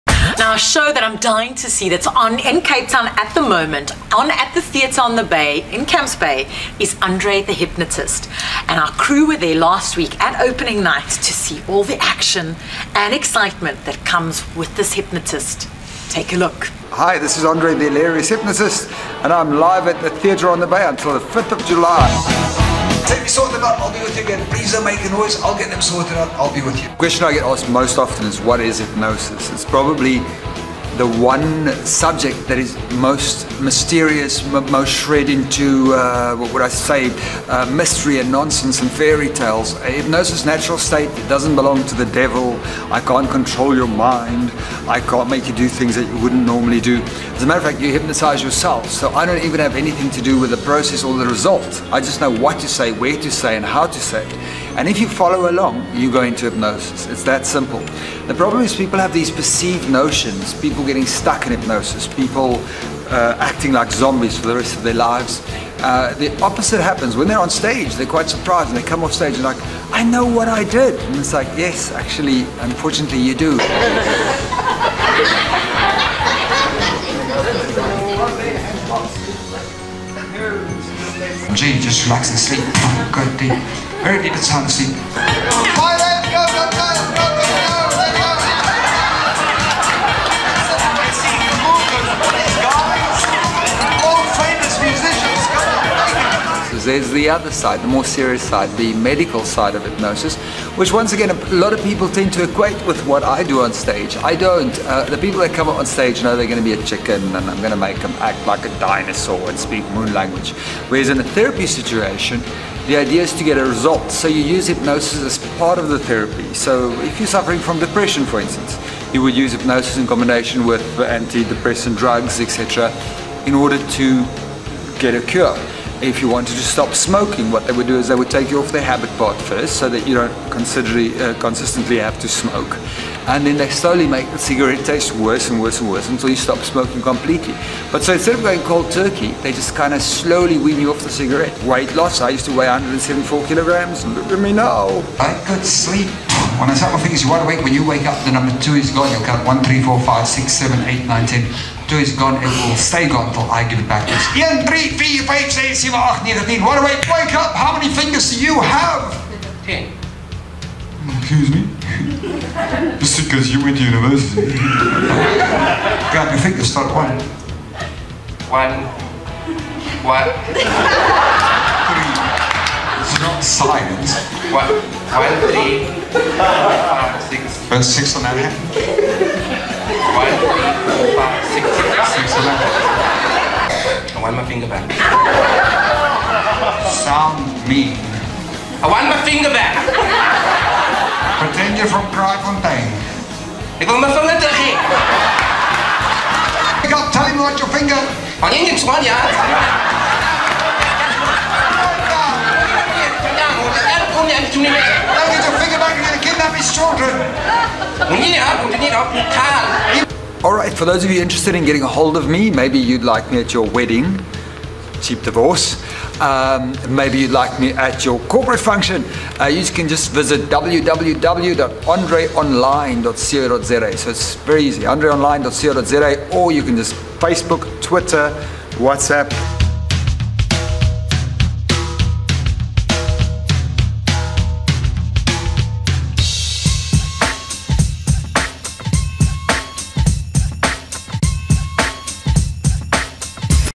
Andre the Hypnotist at Theatre on the Bay 2014
Andre The Hilarious Hypnotist performs his humorous brand of hypnosis in a wild and whacky show.

For most South African`s Andre`s show has become an annual institution - a yearly must-see fix of fun as he travels across the country in similar fashion to his mentor and teacher the late "Max Collie".

The award-winning Andre adds his own special brand of humour to his hypnosis show which requires maximum audience participation. He uses delightful physical comedy as he calls for volunteers to be hypnotized.